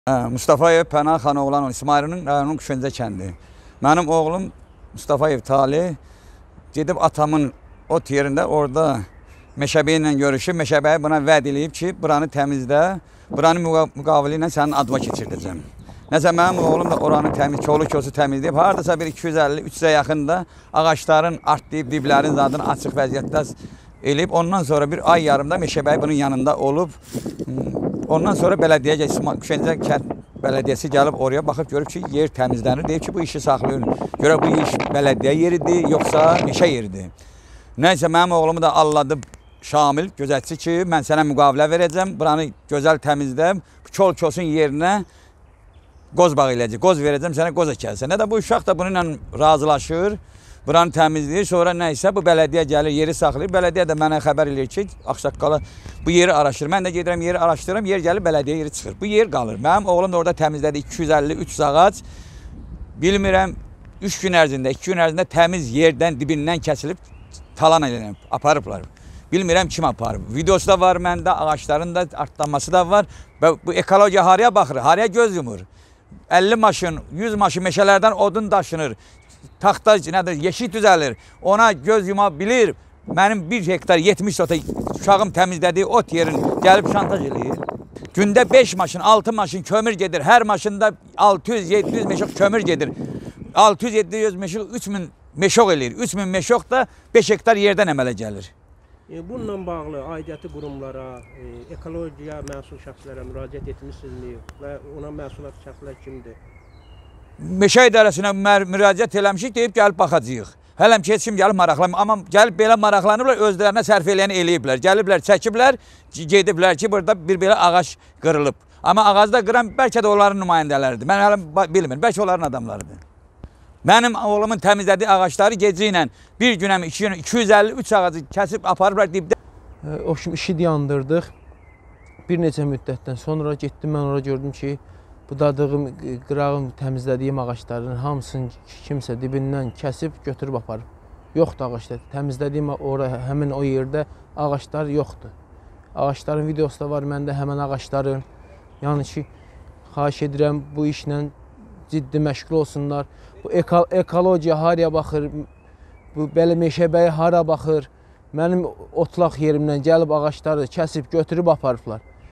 İsmayıllıda ekologiya və bələdiyyənin torpaq davası Zərərçəkən isə sadə kəndlidir
İsmayıllı rayonu Quşencə kəndi ərazisində Mustafayevlər ailəsi illərlə istifadə etdiyi otlaq sahəsini meşəbəyinin təklifi ilə bağa çevirmək istəyib. Ərazi kol-kosdan təmizlənib. Qoz əkmək məqamı gələndə bələdiyyə bildirib ki, torpaq sahəsi onlarındır. Arada Mustafayevlərin zəhməti hədər gedib. Kol-kosdan təmizlənib təzəcə azadlığa çıxmış cavan ağaclar isə gecə ilə kəsilib. Müstafayevlər ekologiyanın belə əməllərə göz yummasına da toxunub.